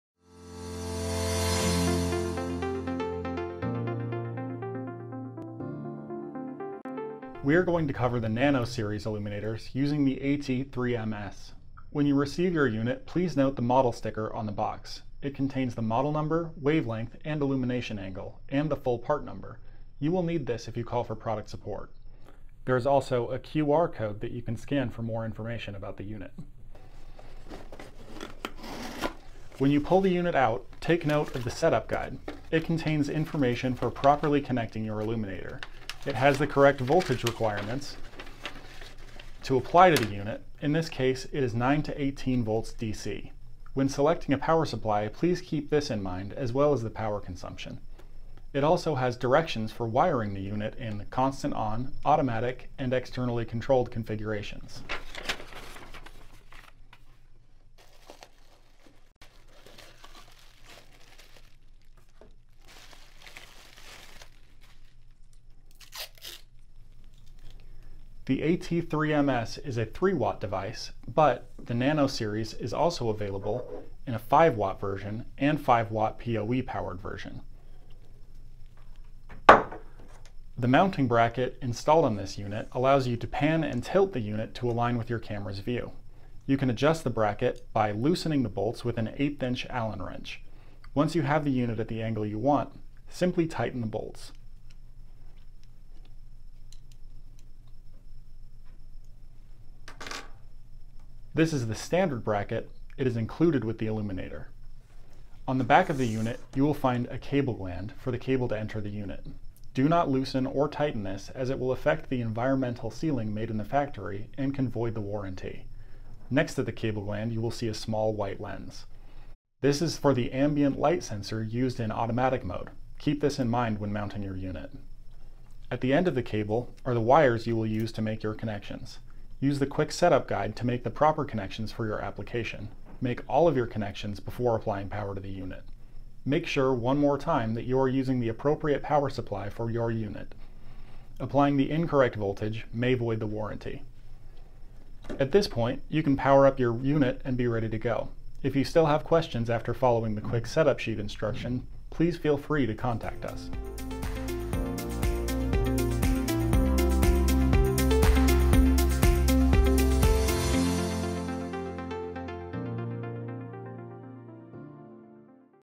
NANO IR Illuminators - Unboxing & Setup Guide Basics
AXTON NANO Series IR Illuminators

Unboxing NANO IR Light Illuminators; Bracket Adjustment and setup guide basics. Nano Series Infrared Lights are Outdoor Rated IR Illuminators designed for Security System Cameras. Nano is available in 850nm IR and covert 940nm IR models.